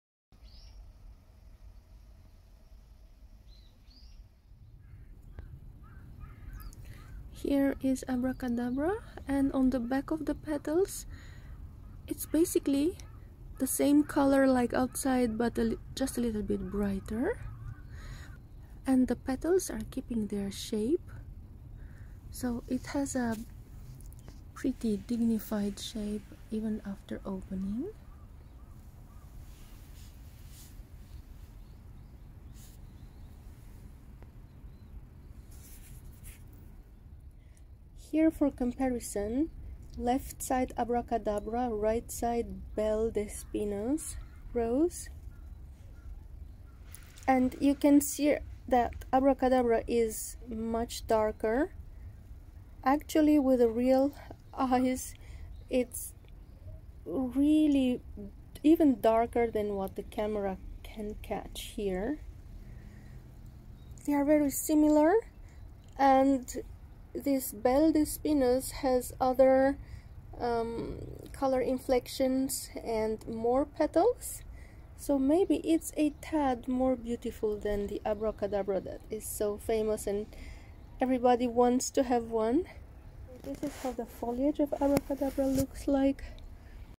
ABRACADABRA ROSE plant Floribunda Discovered by KORDES 2001 Thornless Variegated Yellow Red stripes
one of the most Wanted roses, abracadabra was discovered by the German breeders KORDES & Sons before 2001. it is a very dark red maroon with bright yellow lemon variegation and the pattern shifts so it is possible that it ends up in only yellow or only red flowers the variegation is unstable. not too tall up to 80 cm it blooms in flushes through the season medium double flowers up to 25 petals almost thornless with matte dark green foliage none to mild fragrance. it is similar to Belle d'Espinous but a much darker color and shorter grows also the back of the petals are not white. I will film it again when it makes other flowers and add them to the collection.